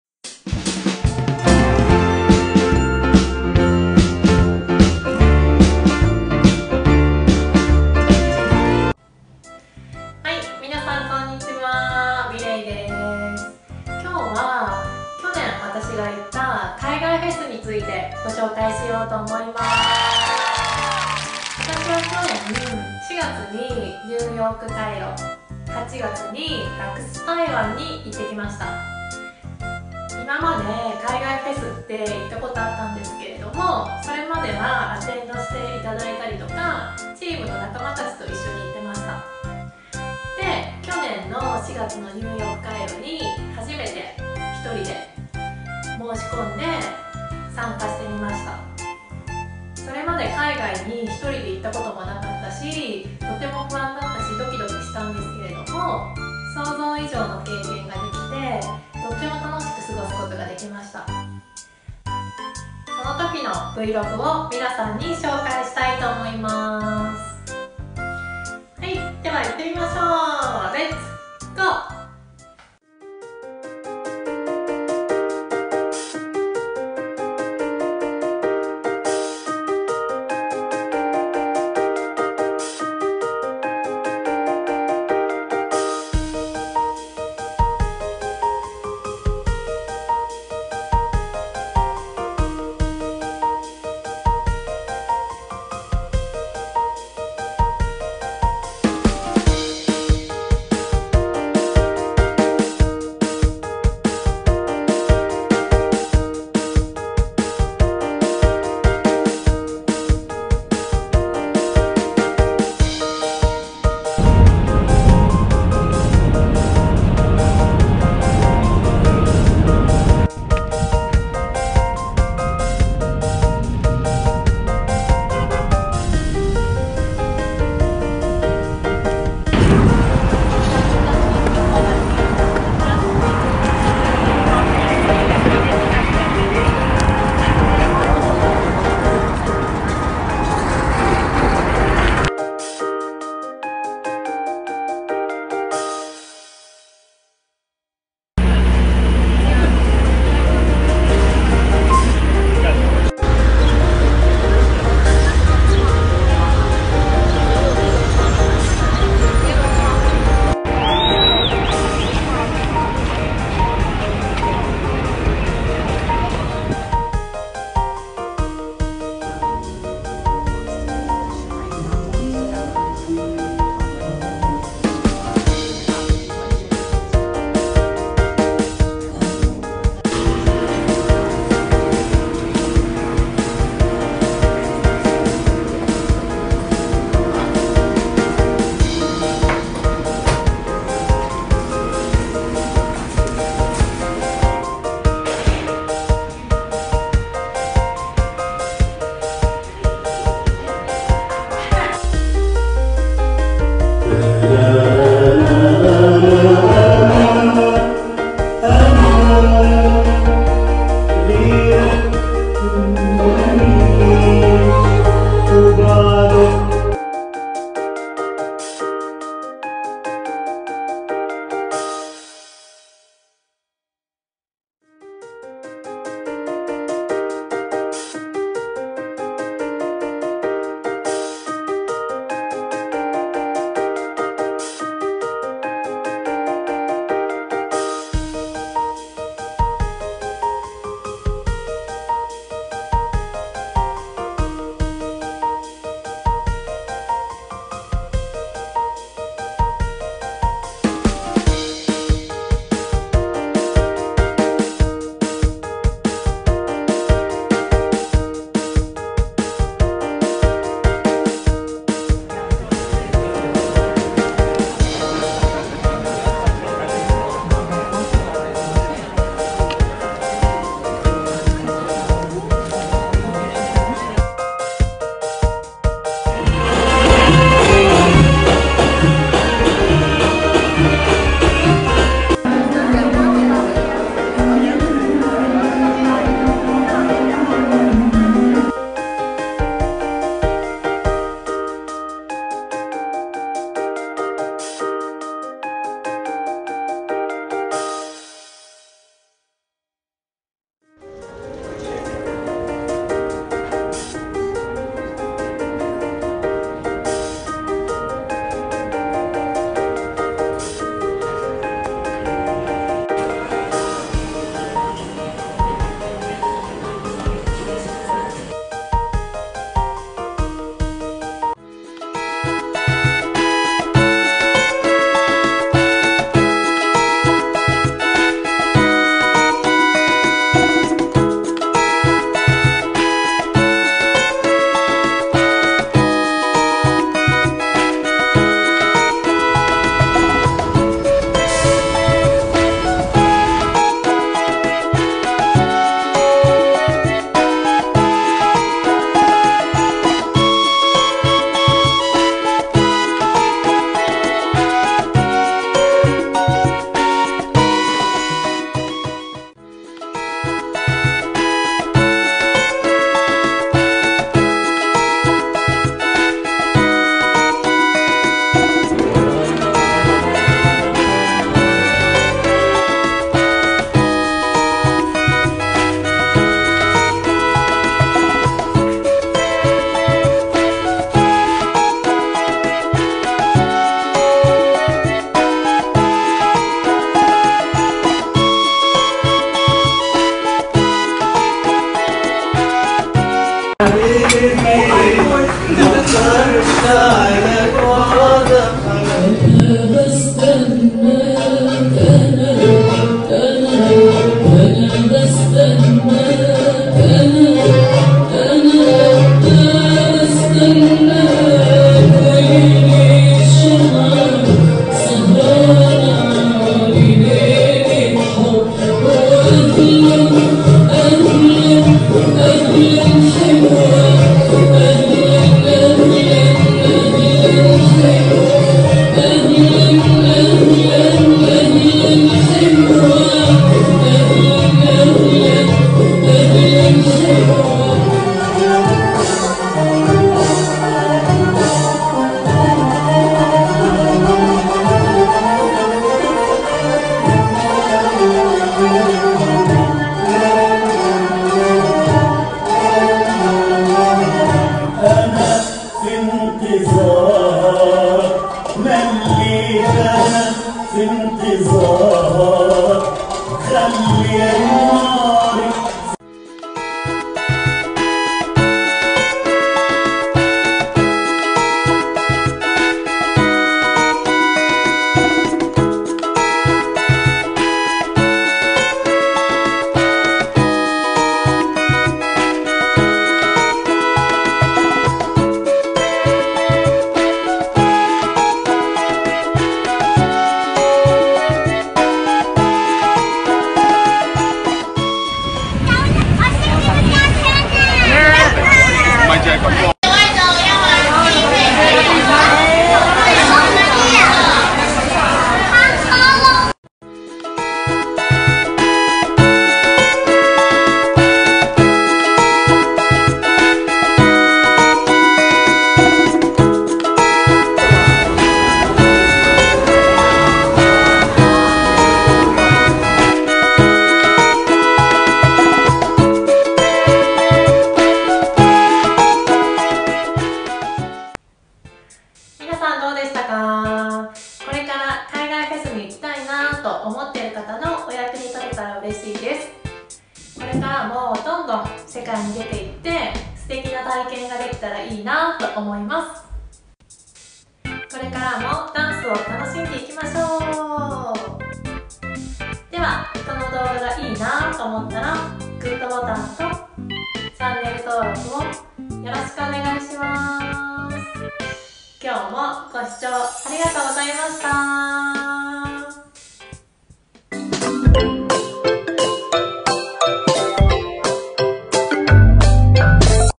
【Vlog】ベリーダンサーMirey はじめてのNY&台湾！/私の海外フェスを楽しむ方法♫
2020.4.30

ベリーダンサーMireyの海外フェスを楽しむ方法をご紹介します。
私が昨年行った海外フェスはNYCairo&Raqs Taiwan
そしてなんと！NYは初めての一人海外旅行！

事前準備から予約、そして現地で楽しみ方などなど
皆さんの参考になれば嬉しいです(о´∀`о)

海外フェスは本当にエキサイティング！
フェスだけじゃなく現地での食事や観光、ショッピングにコミュニケーションも楽しみの一つ。今は便利なツールが沢山あるので、それを活用して安全に快適に旅を楽しんでください♫

私も早く次の海外フェスへ旅立ちたい…♡